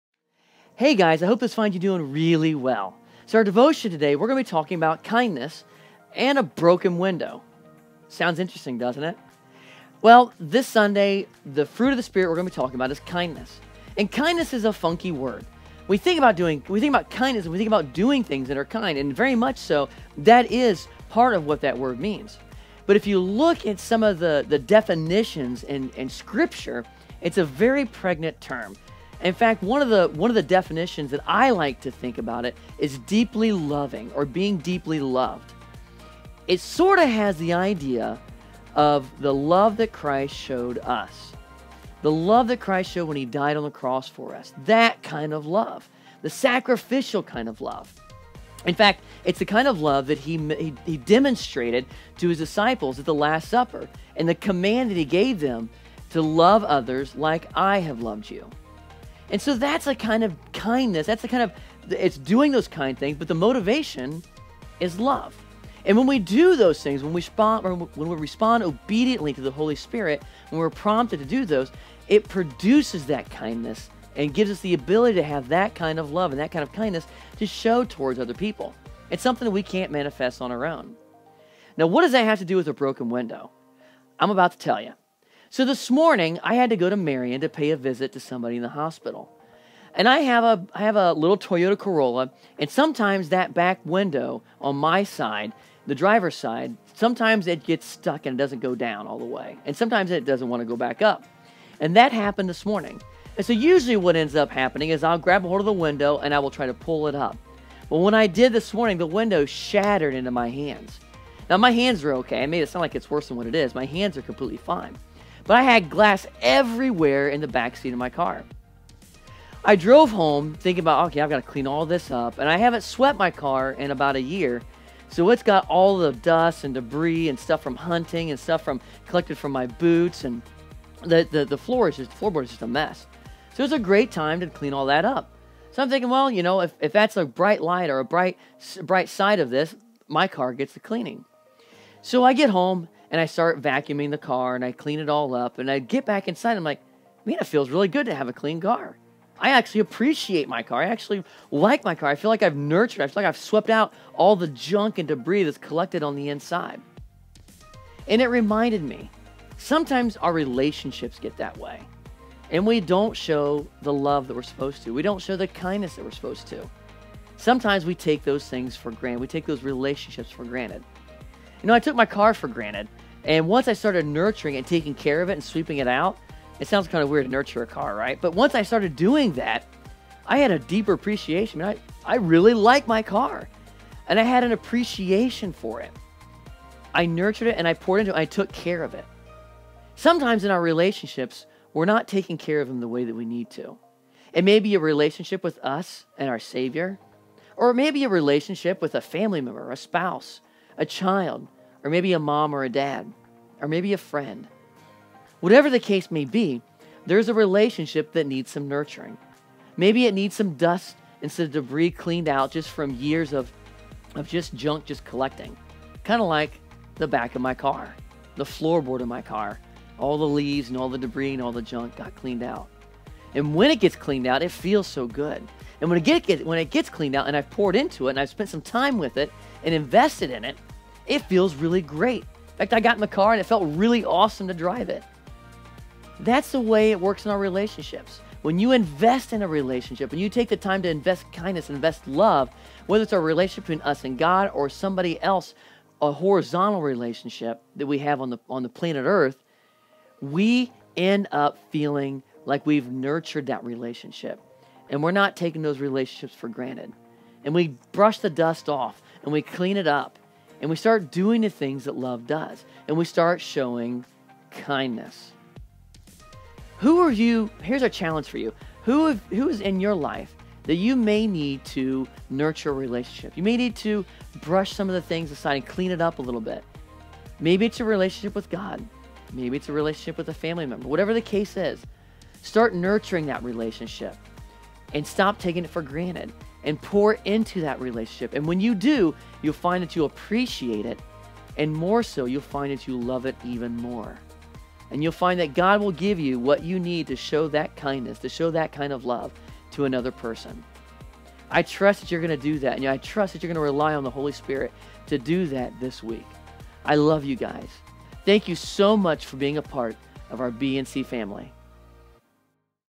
Glass and Kindness
This Sunday we will be talking about the fruit of Kindness!  But what does it have to do with glass?